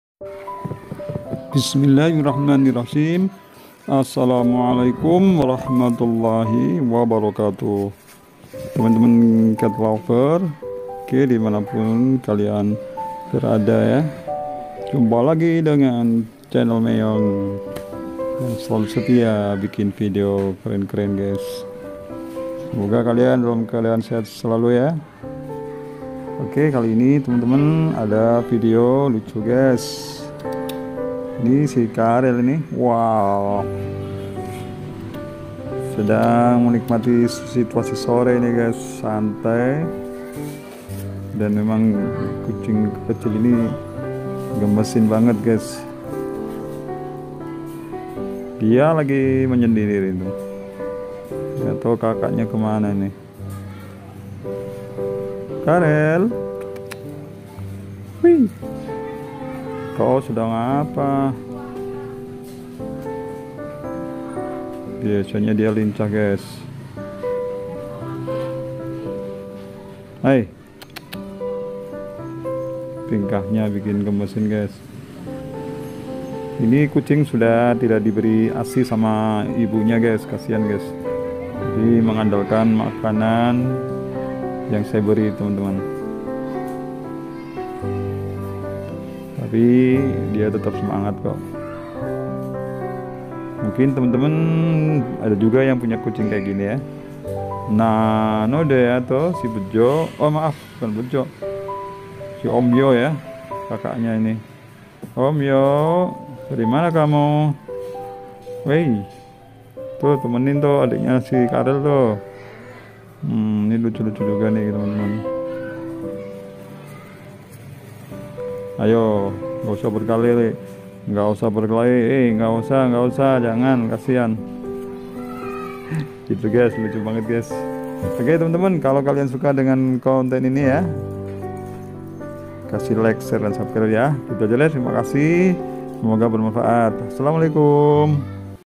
Si Carrel Kucing yang imut dan nggemesin
Si Carrel Kucing yang imut dan nggemesin.Dalam video ini tampak Si Carrel kucing yg imut  tingkahnya bikin gemes @CHANNEL MEONG